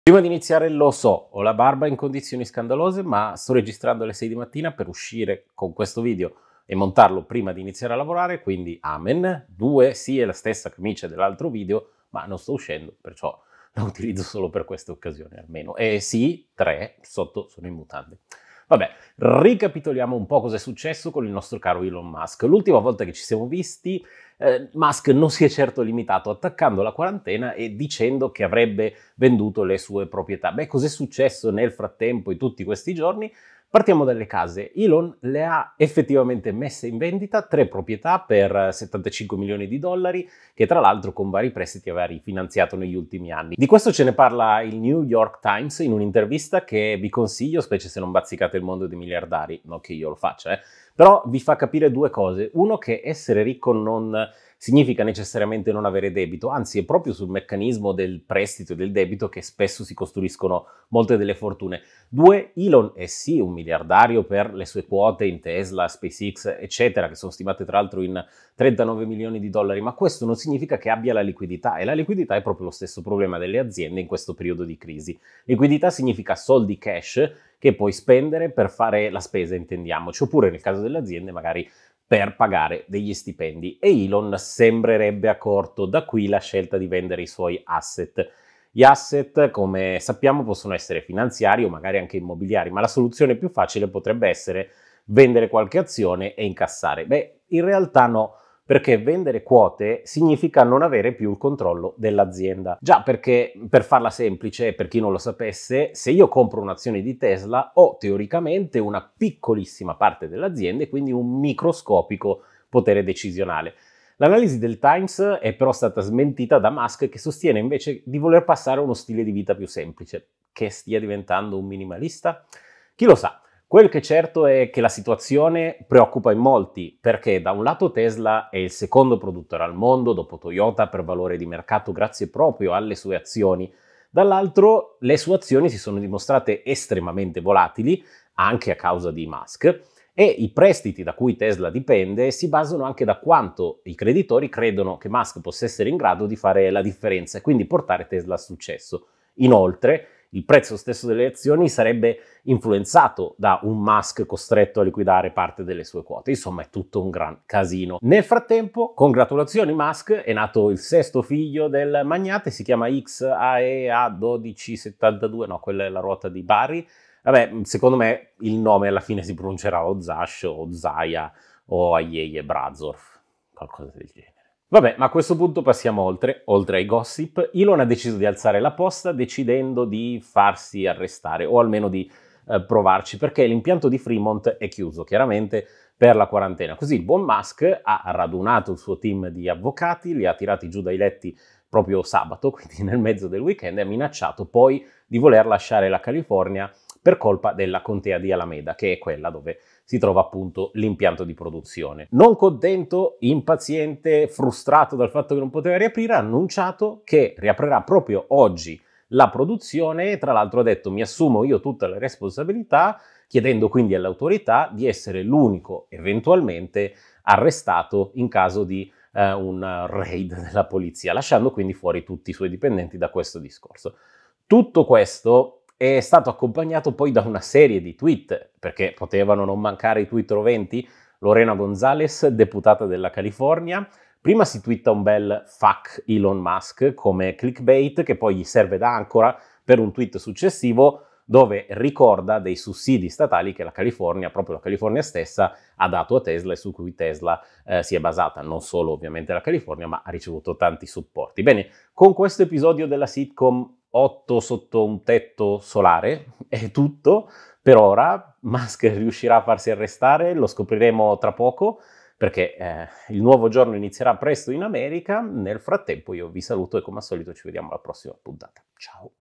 ELON MUSK rischia l'ARRESTO, X Æ A-12 e CASE in VENDITA
Elon Musk insiste per l'apertura dell'impianto 🚗 Tesla 💡 di Fremont, sfida la Contea di Alameda in California, litiga su Twitter con una deputata e...accoglie il suo sesto figlio: X Æ A-12 Musk

Crisi di liquidità per il volto di Tesla?  💲 Le ipotesi del Wall Street Journal nell'intervista che tratta anche delle proprietà immobiliari messe in vendita da Musk per 75 milioni di dollari 🏡